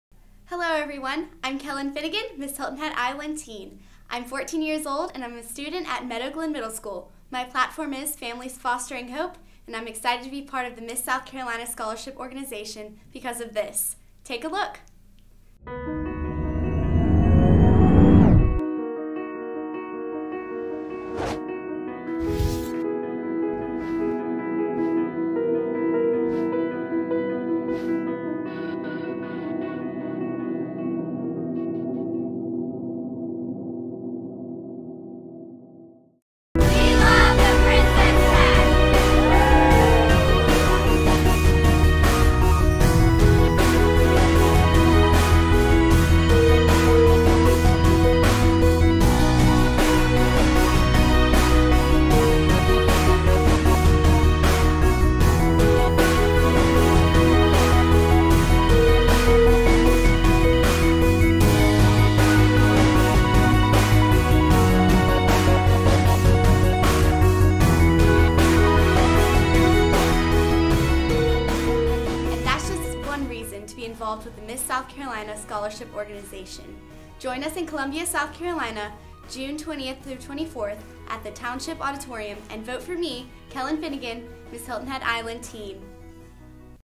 Hilton Head Island TEEN - Kellan Fenegan
The Miss South Carolina Scholarship Organization is proud to announce your opportunity to vote for a Semi-Finalist at the 2017 Miss South Carolina Teen Competition, at the Township Auditorium in Columbia on Friday, June 23, 2017. We have posted videos made by each of our 2017 contestants, showing off their personality and explaining why they are a part of the Miss South Carolina Scholarship Organization!
The contestant with the most votes will be named People's Choice and receive a spot in the Top 16!